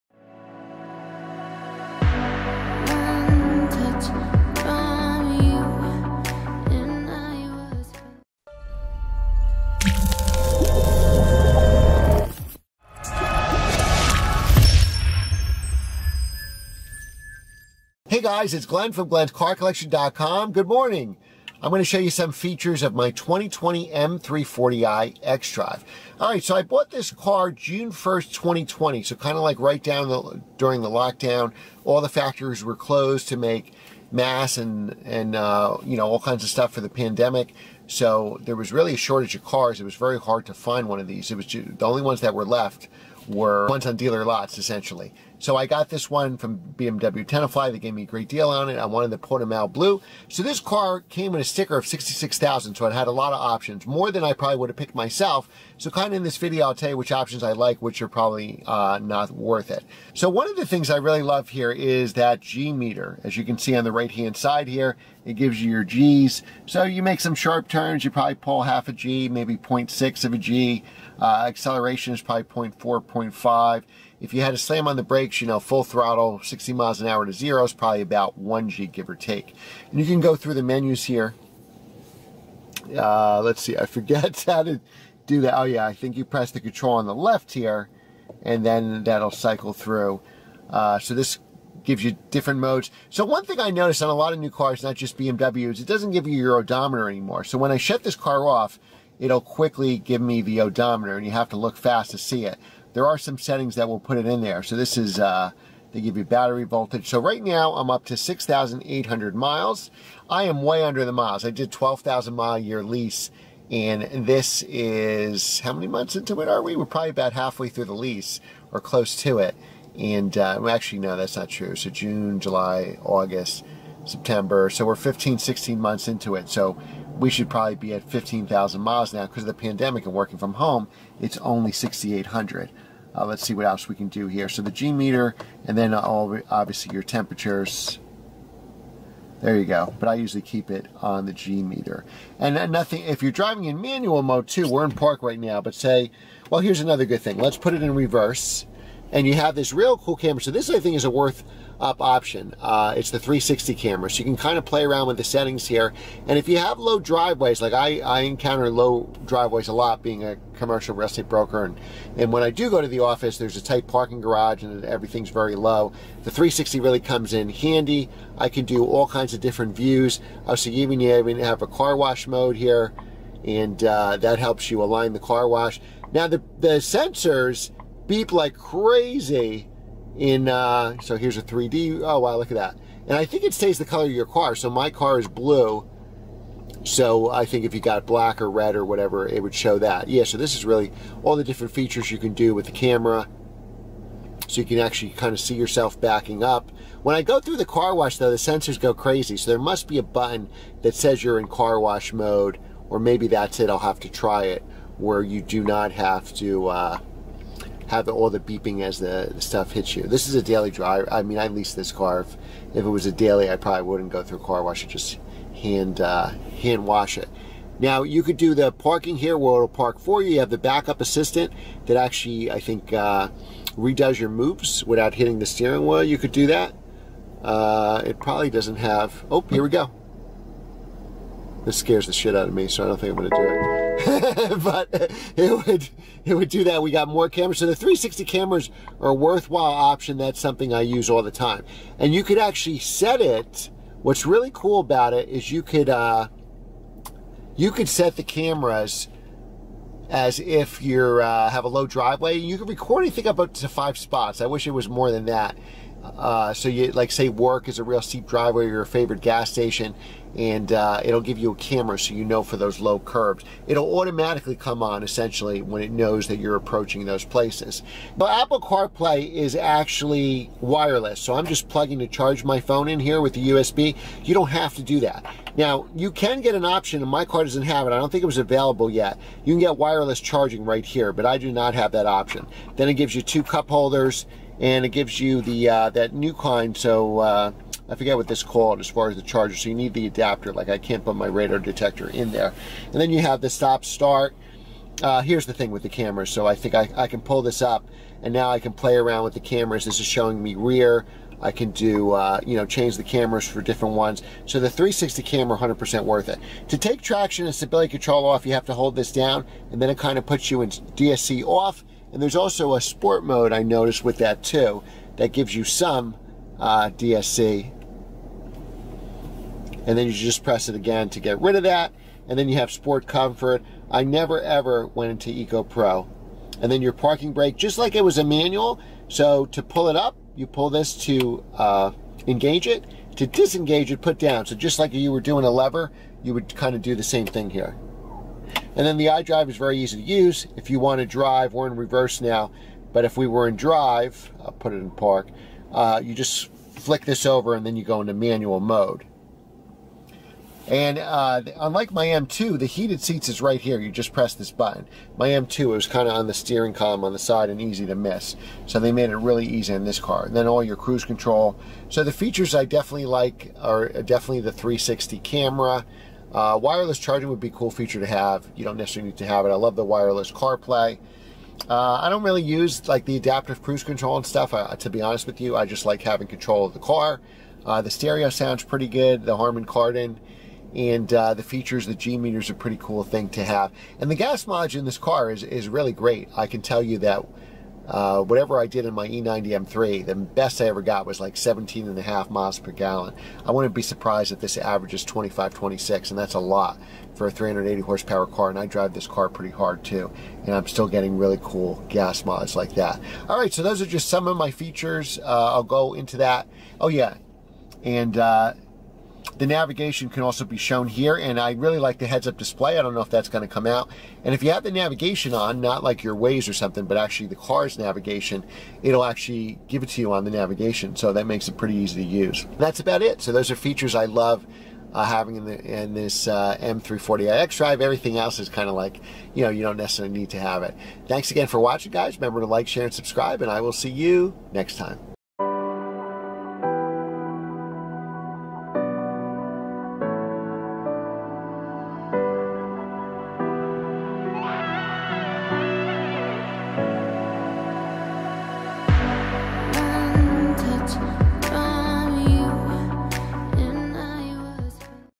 My Favorite Features of the BMW M340i
In this review I go over my favorite features of the BMW M340i XDrive. I can't believe I have owned my BMW M340i xDrive for over a year. and here are some of my favorite features. Watch this video to the end to see if my favorite features of the BMW M340i xdrive.

Glenns Car Collection

Glenns Car Collection

Music

Make sure to join M Gruppe for all BMW M car owners